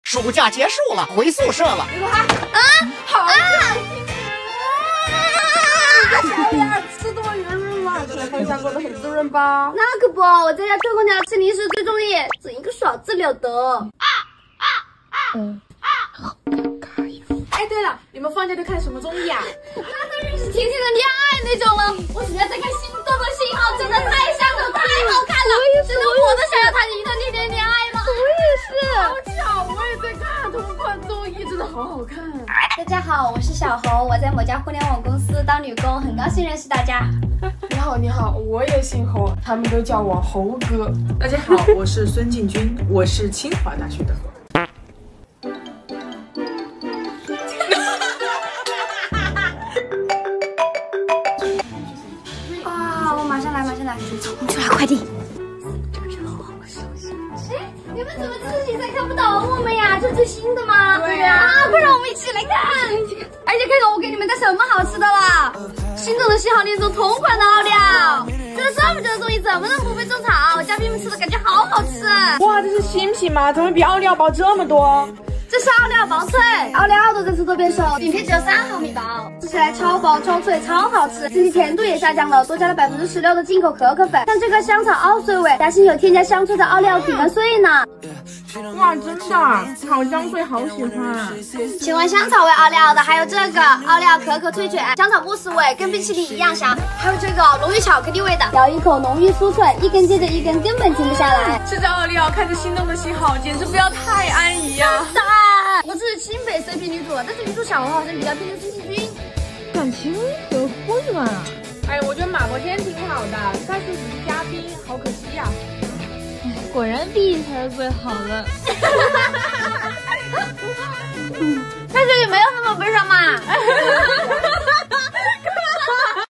Sometimes, all it takes to break the ice in the dormitory is a variety show gossip ~ , female coll
Sometimes, all it takes to break the ice in the dormitory is a variety show gossip ~ , female college student, heartbeat signal, Oreo, Oreo crispy